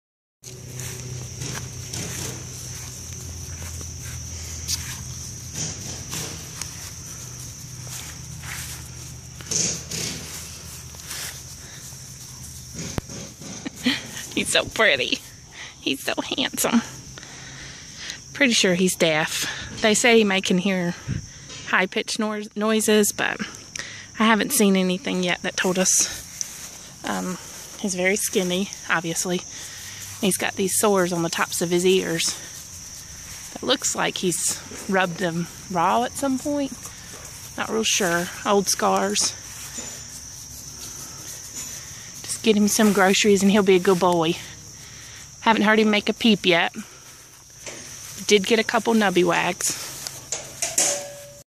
Bam Bam day one
Bam bam pulled from shelter this morning.